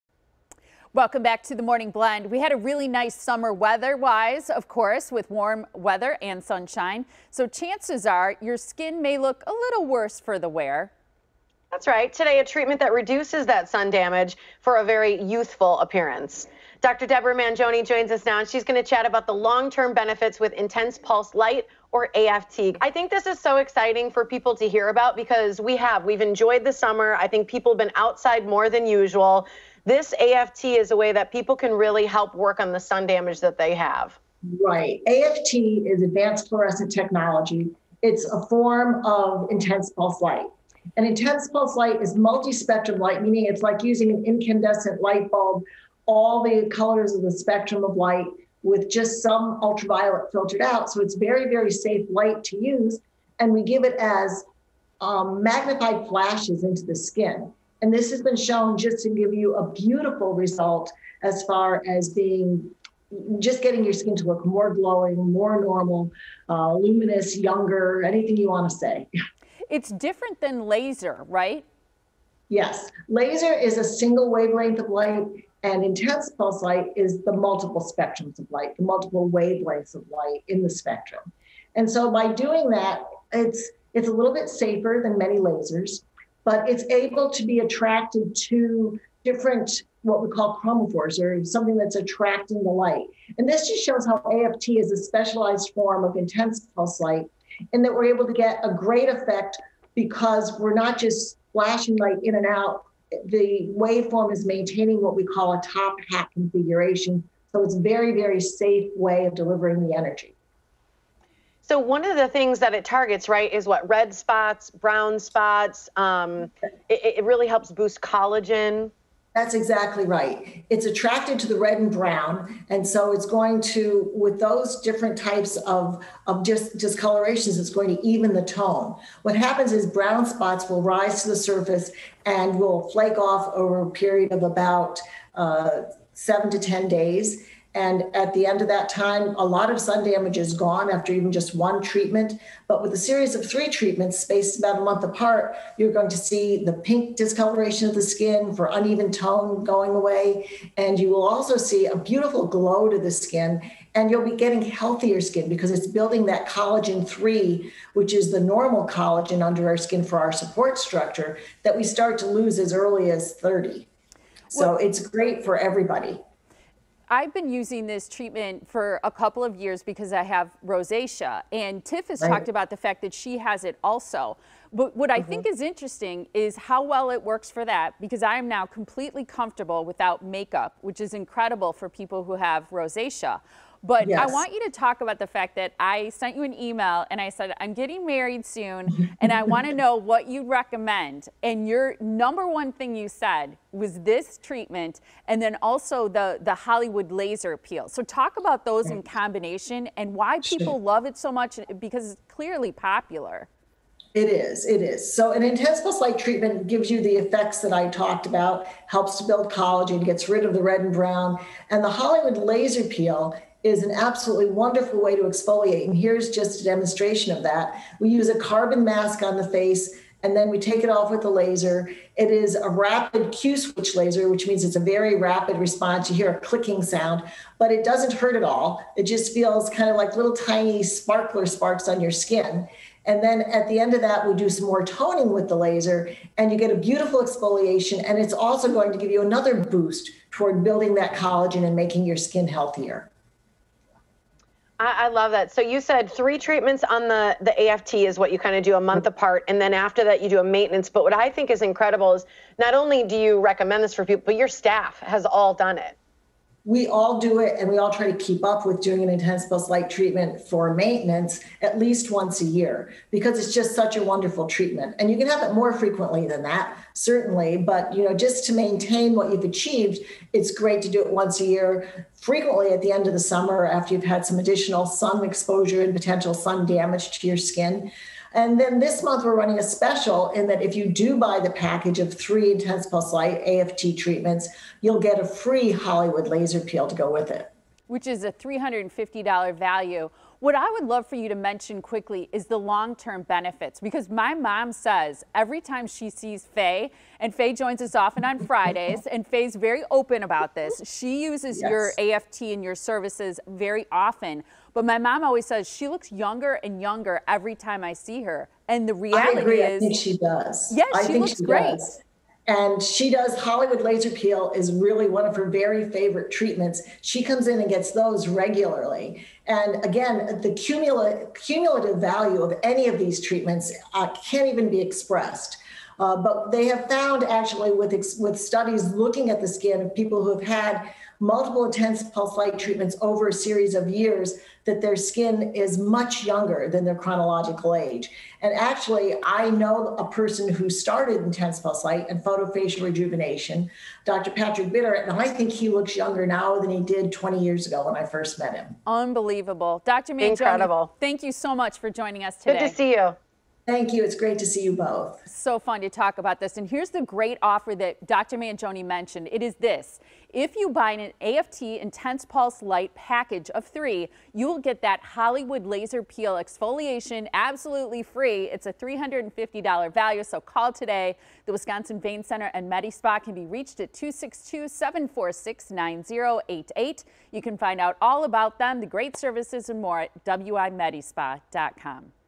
Feel Like Your Best Self with a Safe and Fast Treatment!
One of the easiest ways to erase sun damage and nourish your skin at the same time, is with Intense Pulsed Light, or AFT (Advanced Flourescence Technology). Intense Pulsed Light is different from laser, in that multiple wavelengths of light are delivered to the skin with appropriate filters in place to reduce risk of burn injury, The use of this broad spectrum light has been shown to be a fabulous way to not only clear away redness and brown spots, but also to boost collagen to lead to healthier, more luminous appearing skin. The best part is the treatment is safe and fast! Here to tell us more about AFT and the other treatments available to have you feeling your best is Dr. Deborah Manjoney from the Wisconsin Vein Center and MediSpa.  This month, The Wisconsin Vein Center and MediSpa is offering the AFT intense pulsed light package of 3, to include a free Hollywood Laser Peel exfoliation, that's a $350 value!